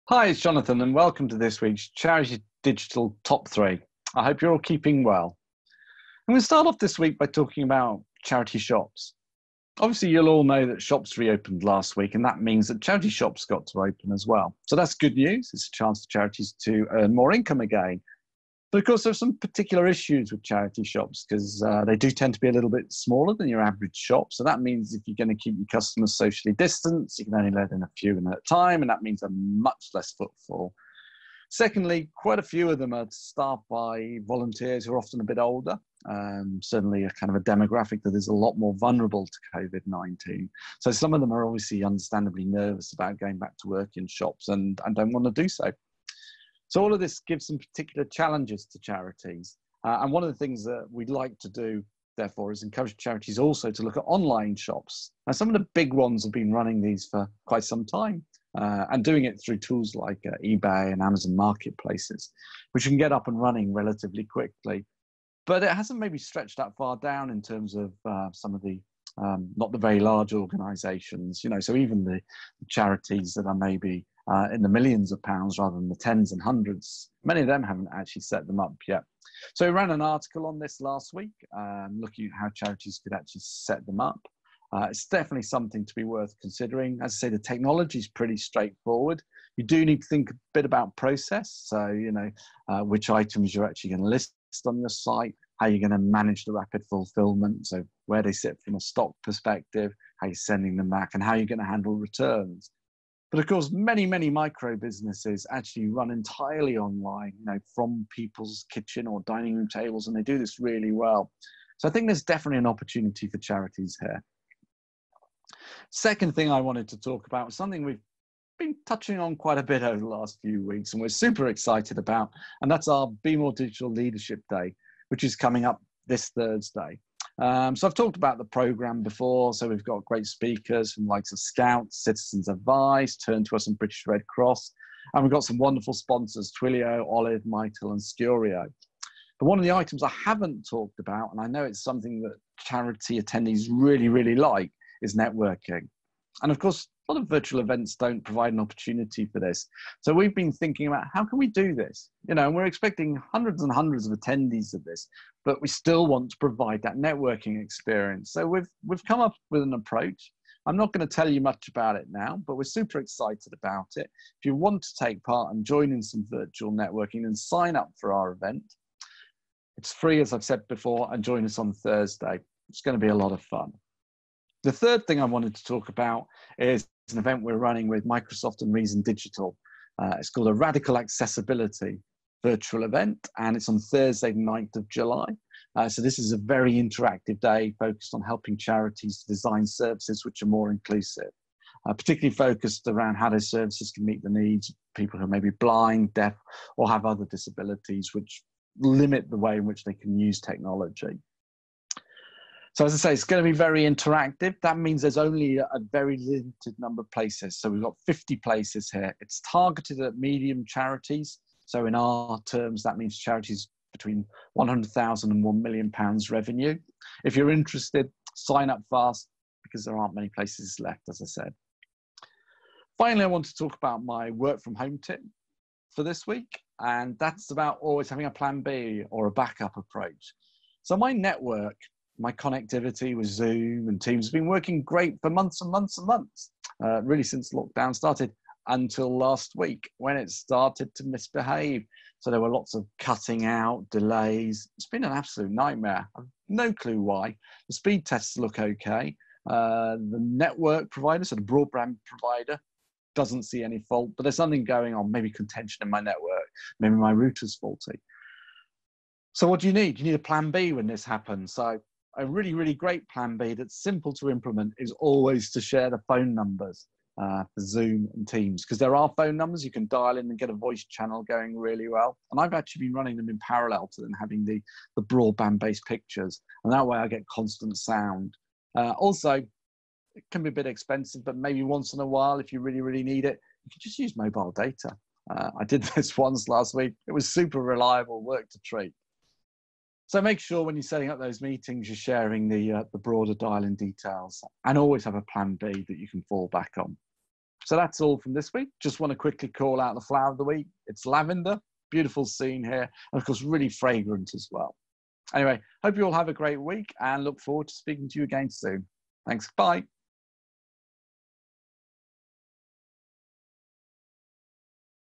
Charity Digital Top 3 Vol.13 with Jonathan Chevallier
This week Jonathan top three covers

- Charity shops
- The BeMoreDigital Leadership Day's networking
- Radical Accessibility with Microsoft and Reason Digital
and as always a work from home tip.